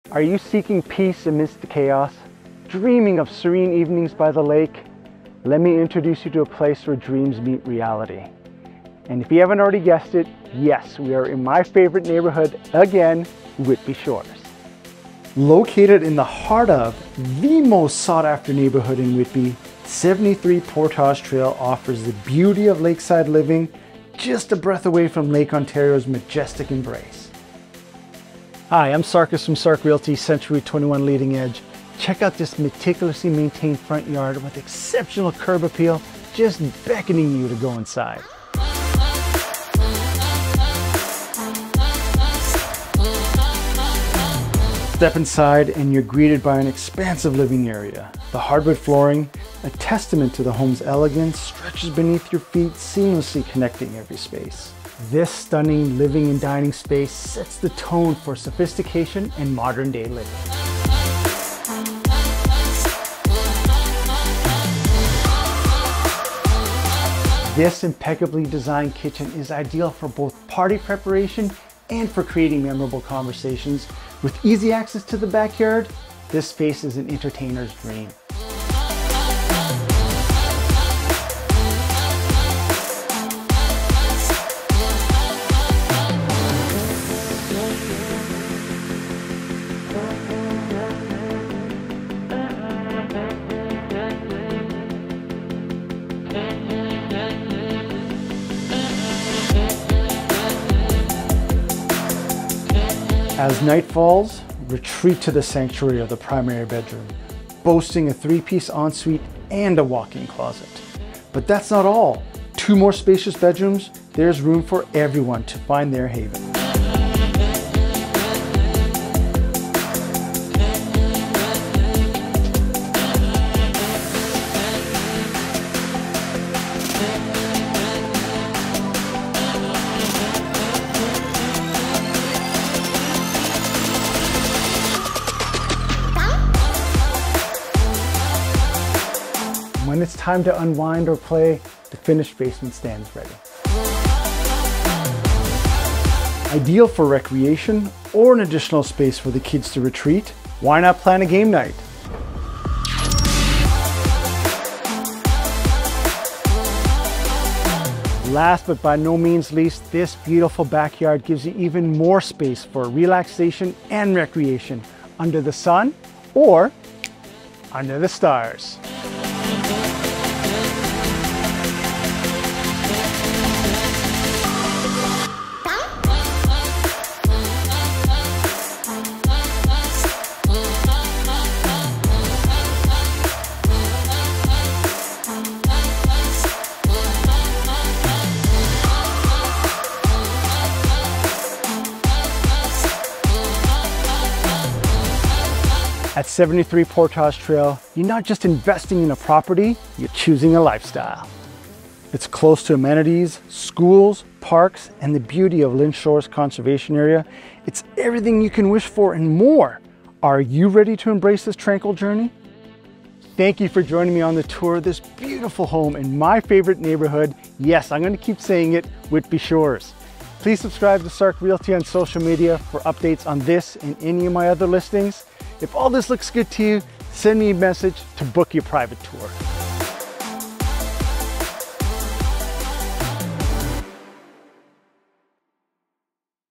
73 Portage Trail, Whitby ON
📍73 Portage Trail, Whitby
📌 $899,900
🛌 Bedrooms:3
🛀 Bathrooms:3

Embrace The Tranquility Of Lakeside Living At 73 Portage Trail, Whitby Shores. This Exceptional Property Boasts 3BD, 3BA & Is Situated Steps From Lake Ontario. A Meticulously Maintained Home With Exceptional Curb Appeal, Invites You To Explore Further.

The Open-Concept Living/Dining Room Sets The Tone For Sophistication & Modern-Day Living. A Sensibly Designed Kitchen Is Ideal For Both Party Prep & Creating Memorable Conversations At The Centre Island + Easy Access To Bkyd. Retreat To The Primary Bedroom With A 3PC Ensuite & Walk In Closet. Two More Spacious Bedrooms Provide Havens For Everyone.

Finished Basement Offers Versatility, Perfect For Recreation, Home Office Or Playroom. Plan A Game Night Or Simply Unwind In This Flex Space. Backyard Boasts A Gorgeous, Expansive Deck, Shaded By Pergola, Gas Fire Pit & Shed.

Extras:Close Proximity To All Amenities, Schools, Parks, Walking Trails, Go Station, HWYS & Natural Beauty Of Lynde Shores. Your Lakeside Paradise Awaits!